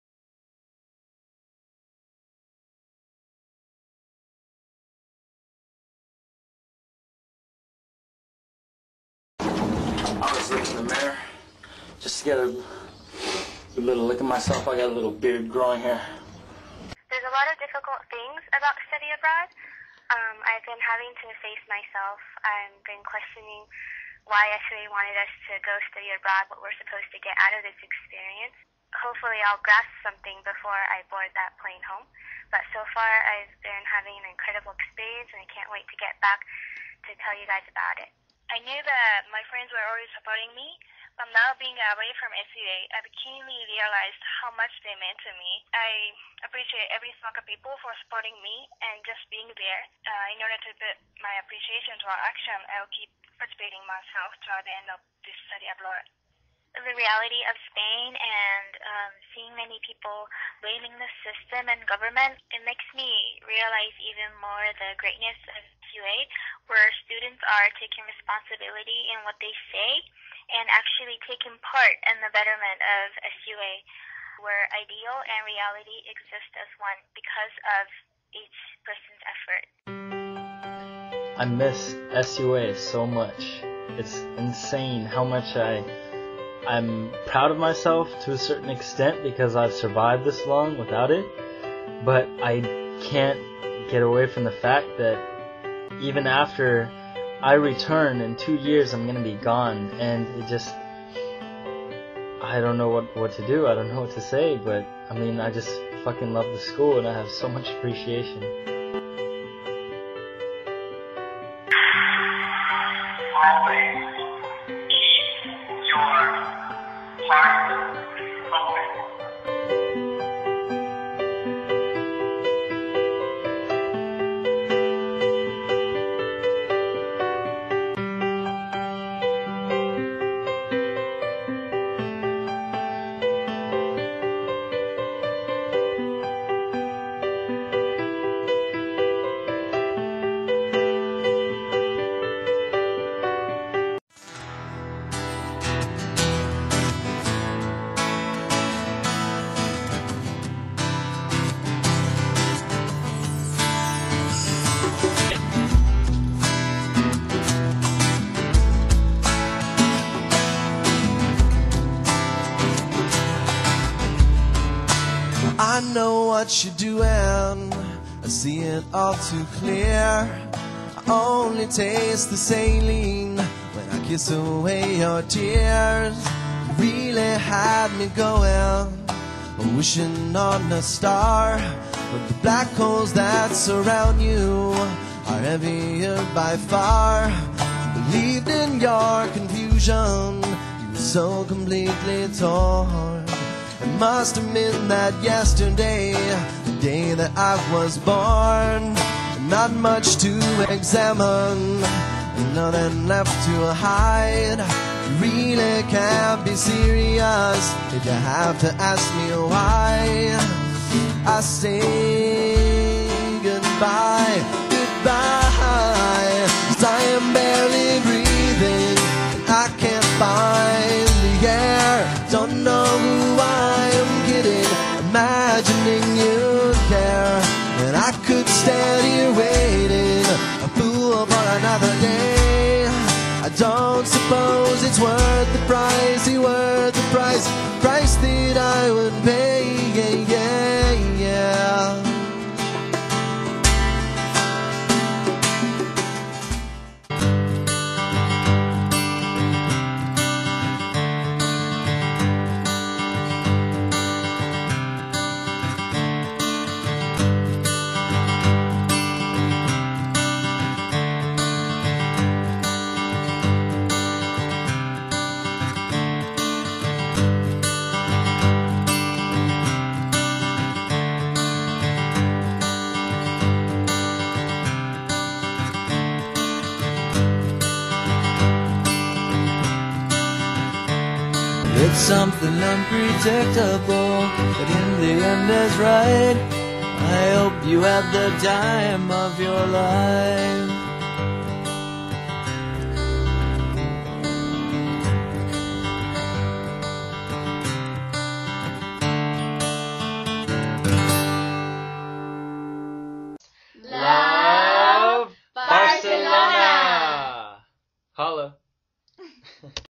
Fall Study Abroad Barcelona 2006
I intended to create this for Student Festival (which is tomorrow), but apparently the video was submitted too late, so I figured I could still show it to people, right? Especially the Class of 2008, oh how miss you! I spent some time on it, it's about 5 minutes (probably too long for the Student Festival anyway), and I hope you all enjoy it!